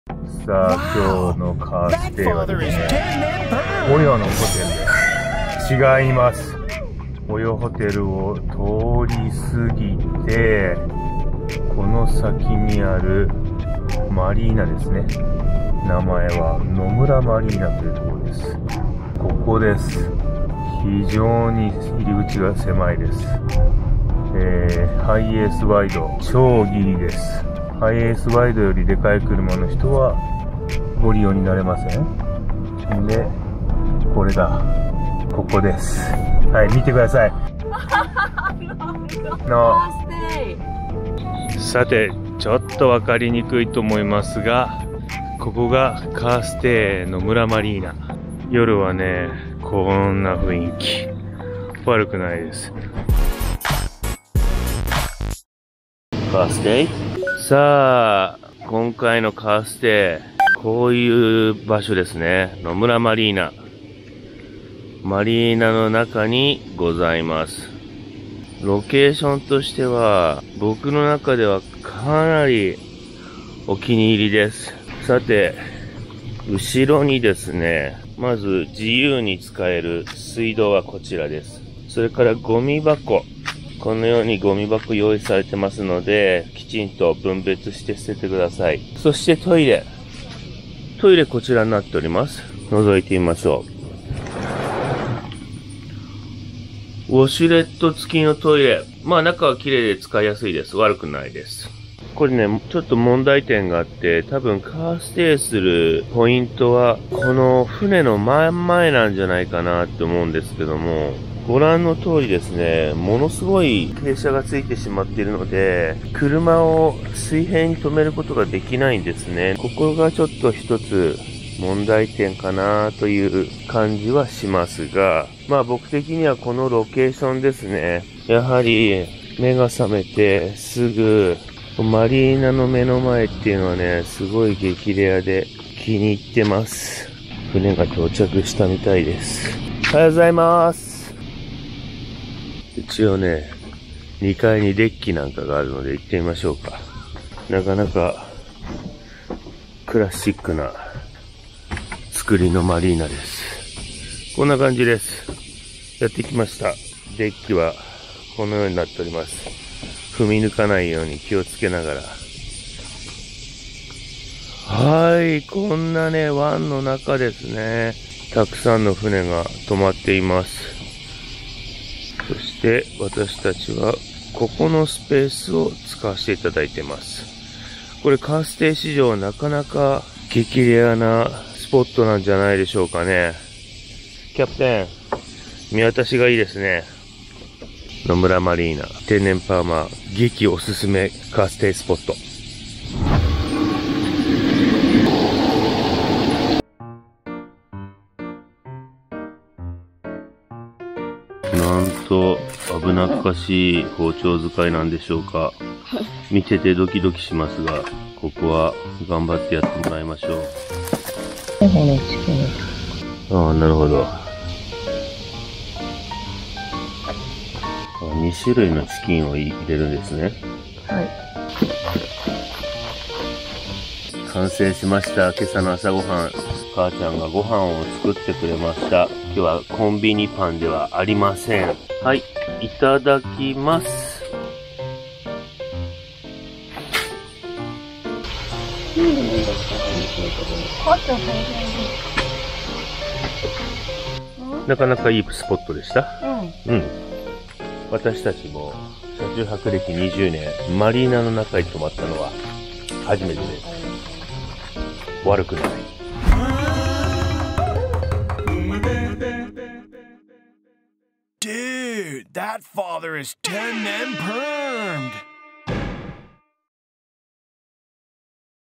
【ハイエース車中泊】マリーナの中で車中泊できるの？？激レア車中泊スポットをご紹介！
三重県の志摩と言えばリアス式海岸で有名ですが、その中の英虞湾にあるマリーナの中で車中泊ができるスポットがあるのです！
日本全国様々な場所で車中泊してきましたが、ここのロケーションは他にない独特の雰囲気！
テンネンパーマ超絶オススメの車中泊スポットです！

Carstayを利用することで快適で安心した車中泊を楽しむことができます。